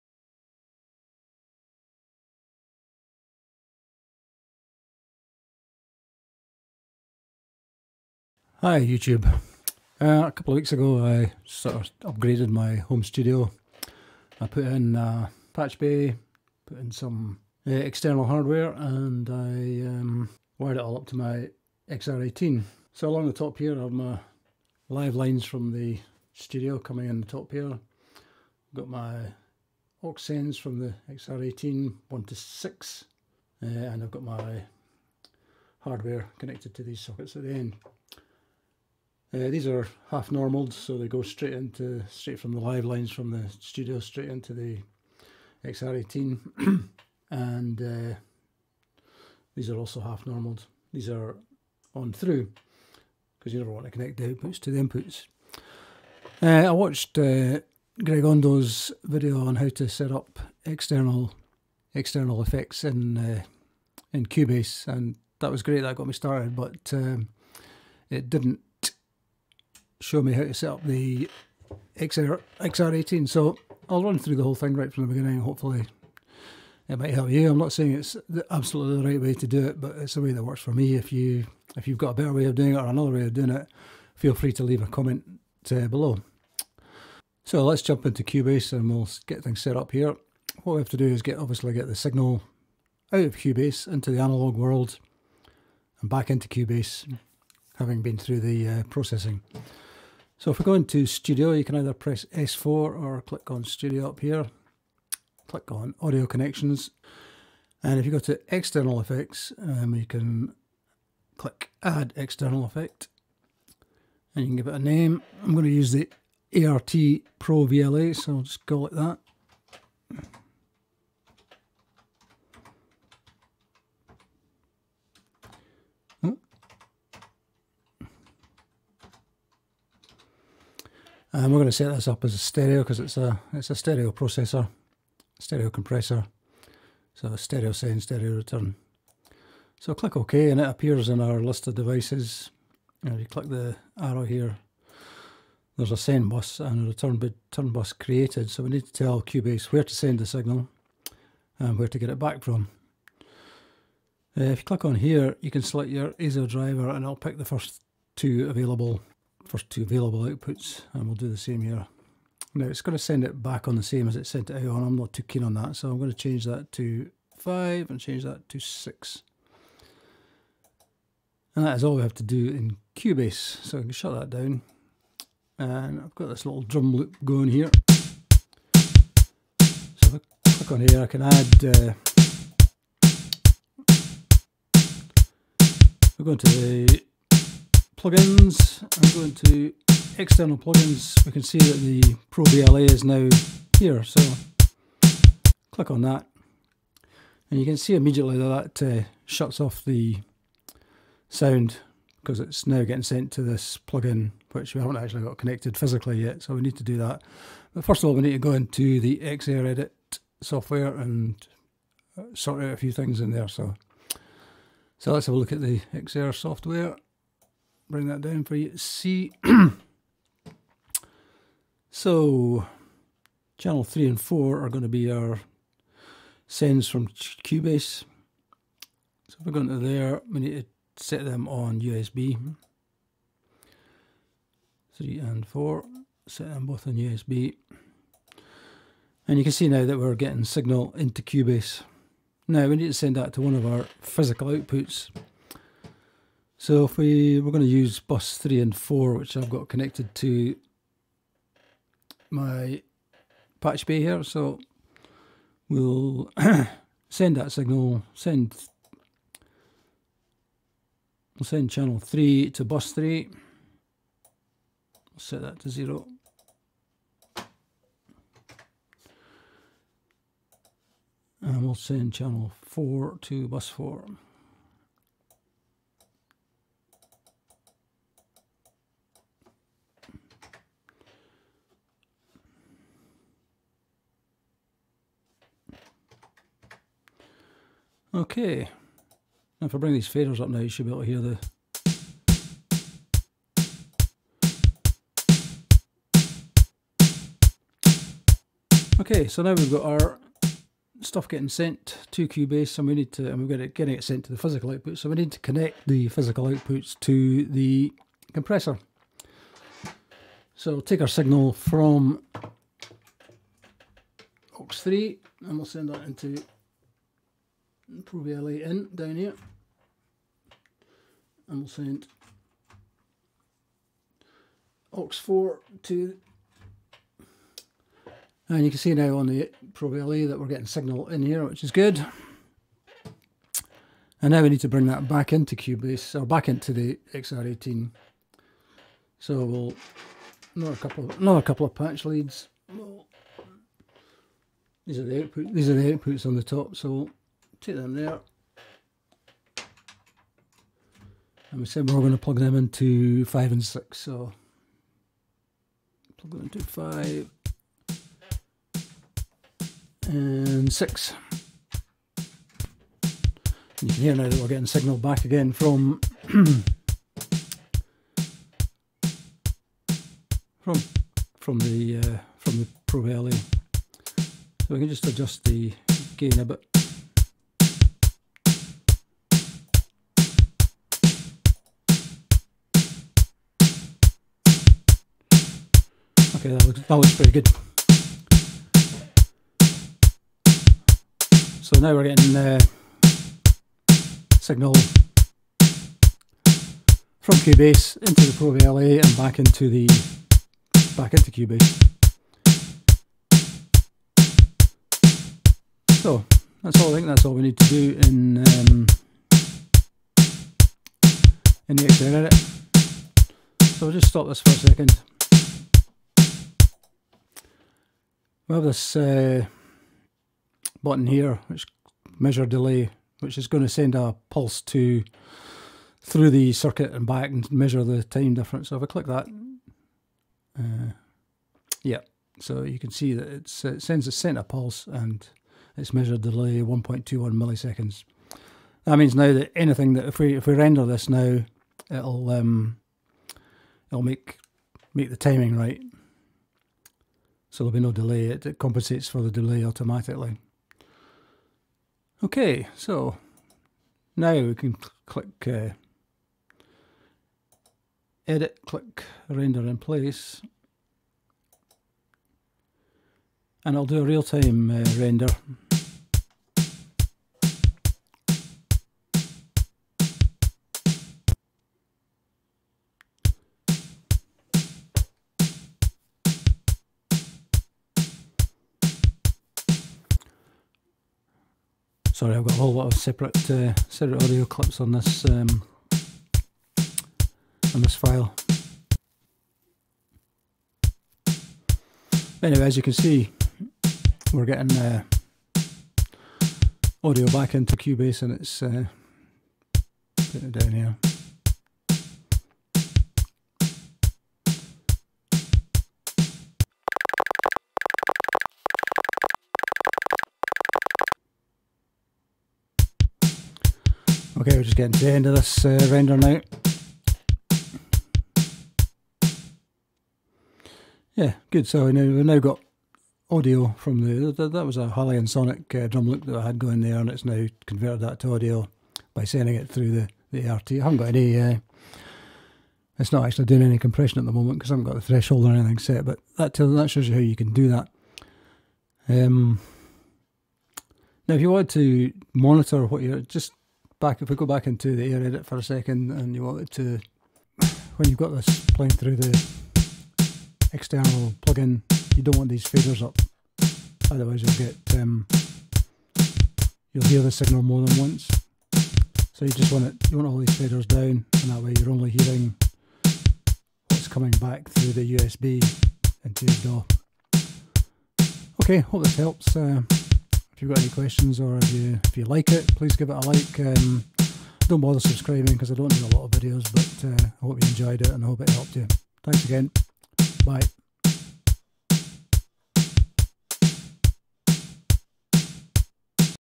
Connect Cubase to External Hardware using XR18
Short tutorial on how to use Behringer XR18 to connect external analogue hardware in Cubase.